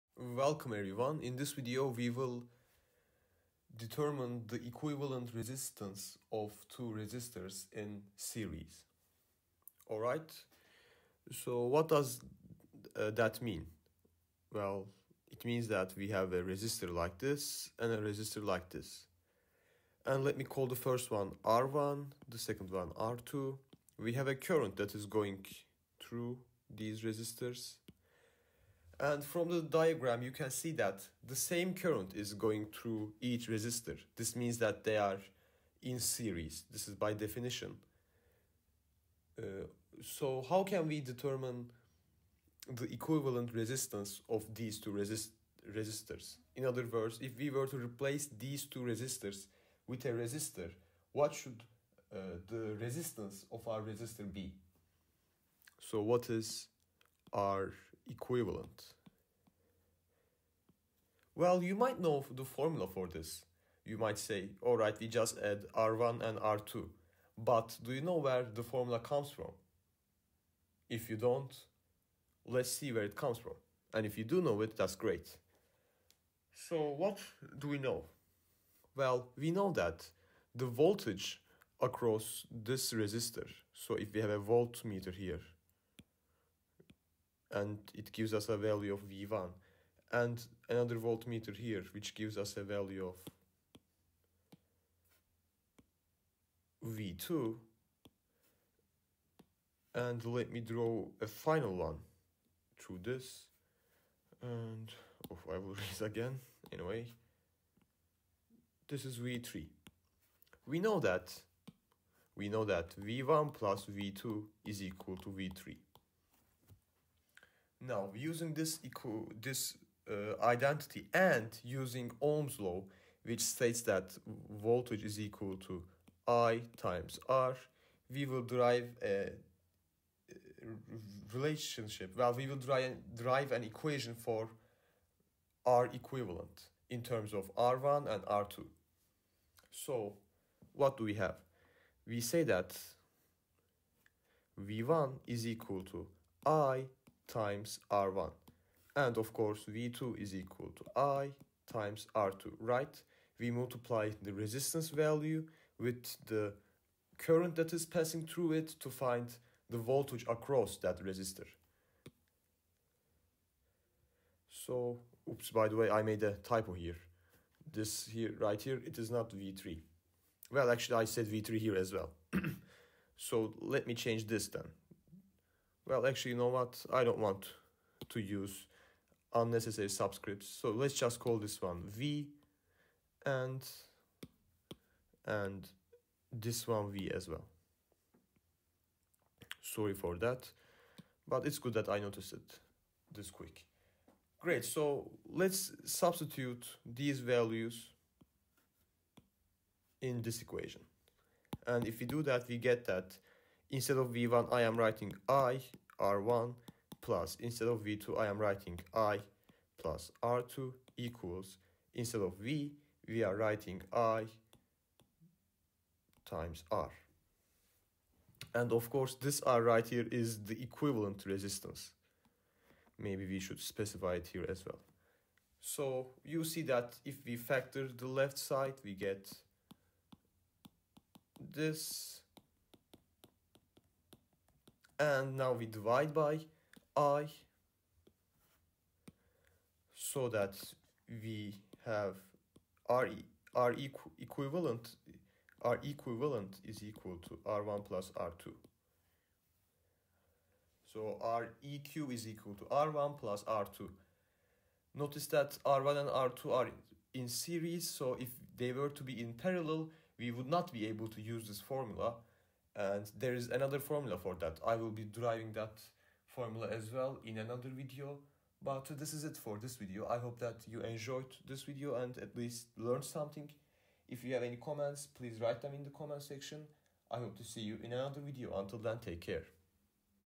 Equivalent Resistance of Resistors in Series
In this video, I derived the formula for the equivalent resistance of two resistors in series.